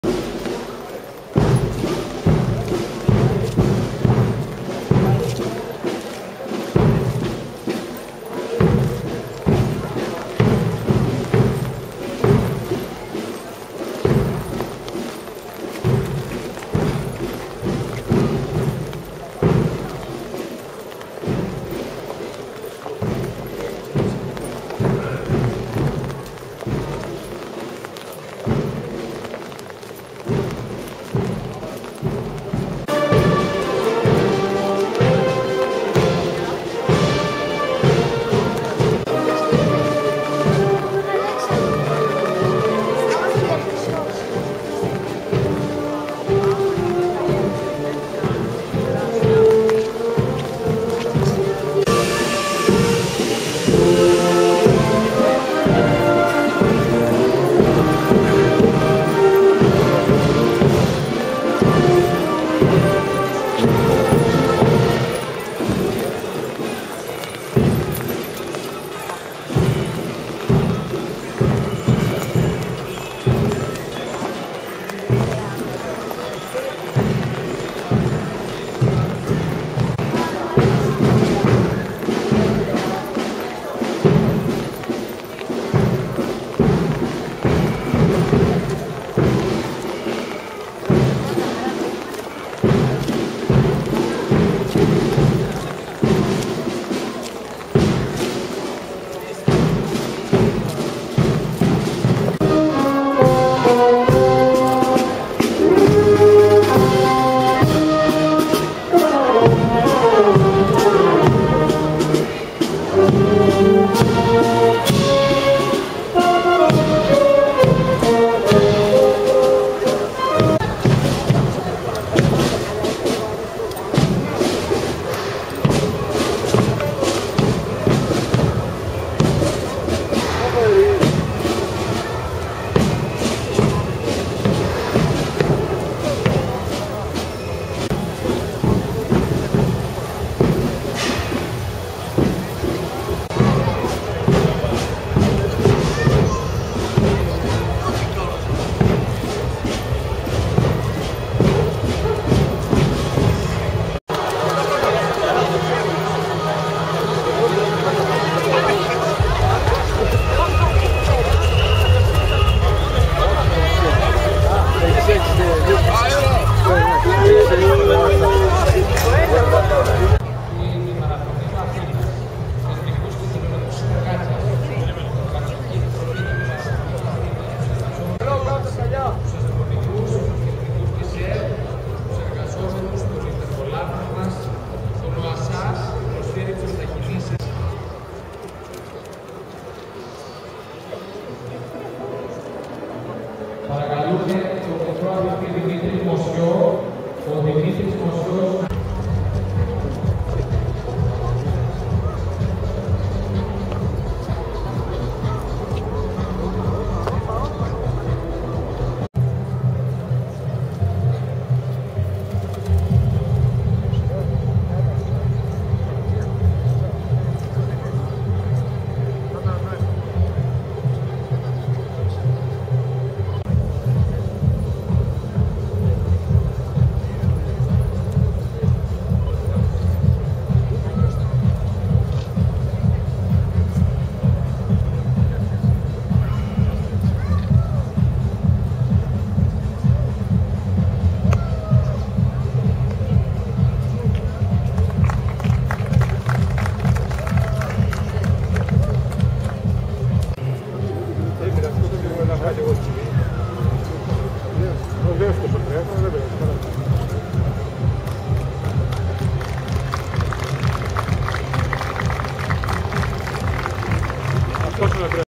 Εορτές εξόδου Μεσολογγιου - Κατάθεση στεφάνων στο Σύνταγμα.
Κατάθεση στεφάνων στον άγνωστο στρατιώτη απο τον Σύλλογο Μεσολογγιτών Αττικής (o Aη Συμιός),τόν Λαογραφικό σύλλογο  Ευηνοχωρίου (ΑΓΙΟΣ ΙΩΑΝΝΗΣ)  και τον σύλλογο Αγίου Γεωργίου για τα 192 χρόνια απο την  έξοδο του  Μεσολογγίου.